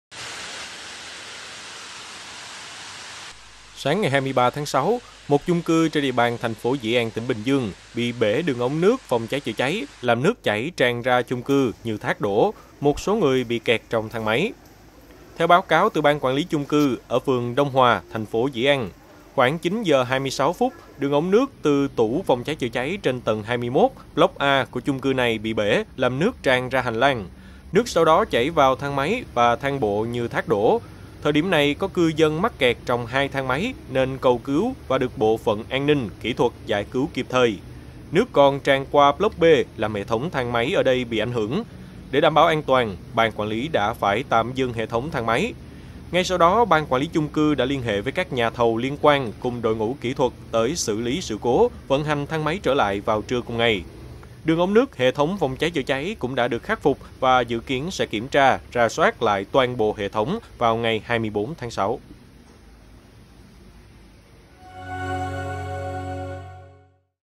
Nước chảy tràn chung cư, nhiều người kẹt trong thang máy ở Bình Dương
Sáng 23-6, một chung cư trên địa bàn TP Dĩ An, tỉnh Bình Dương bị bể đường ống nước PCCC, làm nước chảy tràn ra chung cư như thác đổ, một số người bị kẹt trong thang máy.